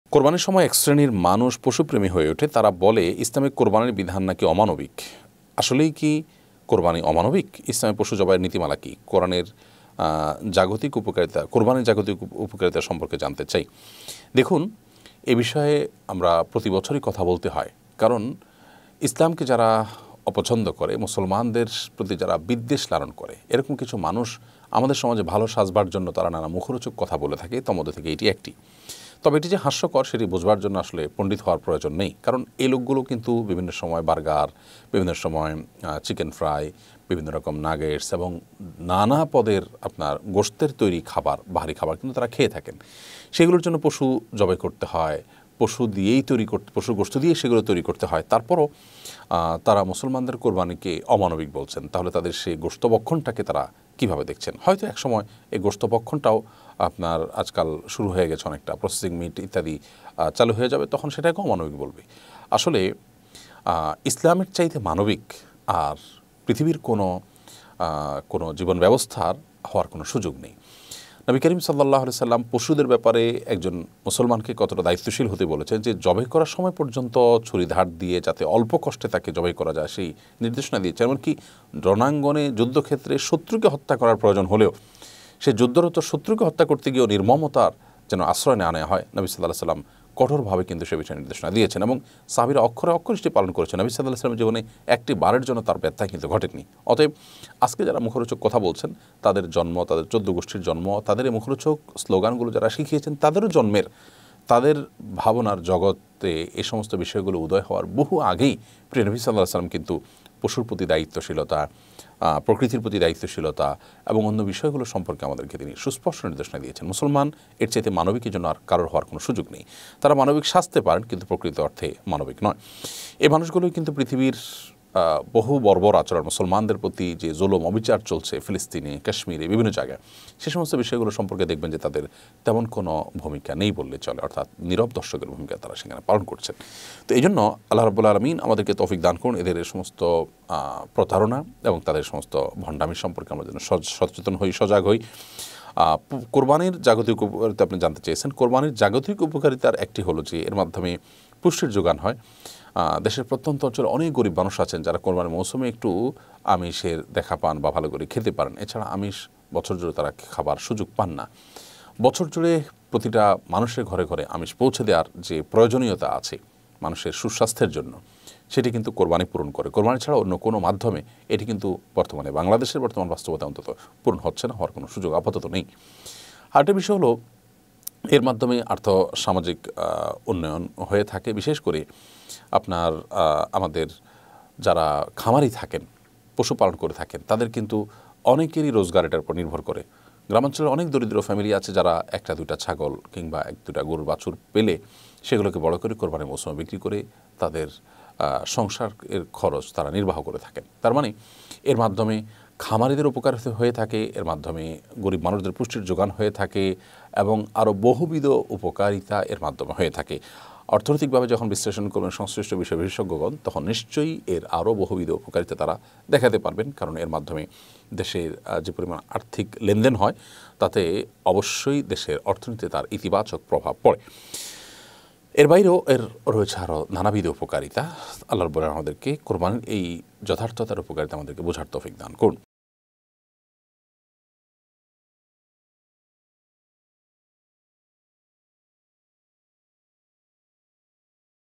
সারাবছর প্রাণীর গোশত খেলেও কু*র*বা*নী*র সময় এক শ্রেণির মানুষ পশুপ্রেমী হয়ে ওঠে এর রহস্য কী?
সারাবছর প্রাণীর গোশত খেলেও কু*র*বা*নী*র সময় এক শ্রেণির মানুষ পশুপ্রেমী হয়ে ওঠে এর রহস্য কী? new waz II waj II Sheikh ahmadullah